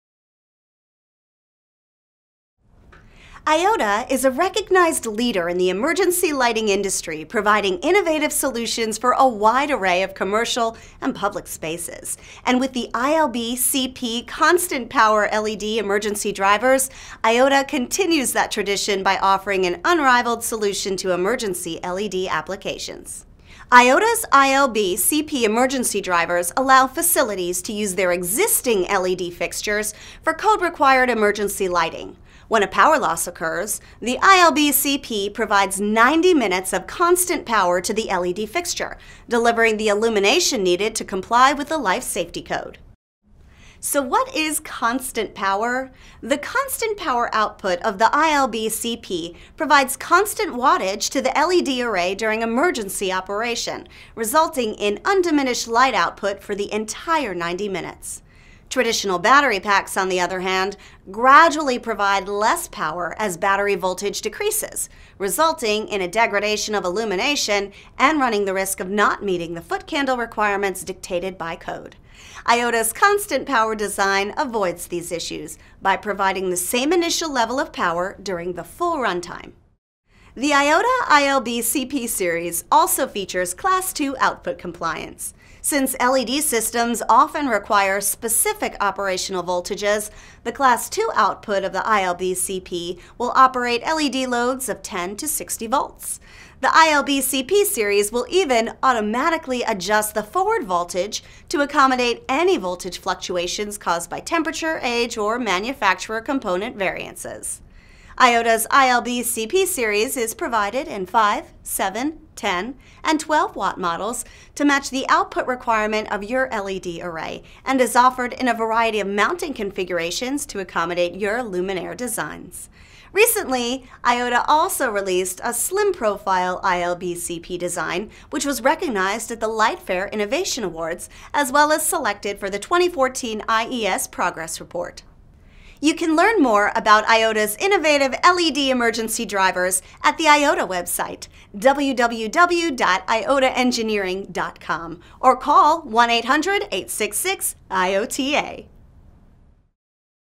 IOTA's Groundbreaking Constant Power LED Emergency Drivers
IOTA's ILB-CP Constant Power LED Emergency Drivers are the first LED emergency drivers to be UL Classified for field installation. The innovative constant power design allows for unrivaled and predictable performance for a wide array of LED emergency applications.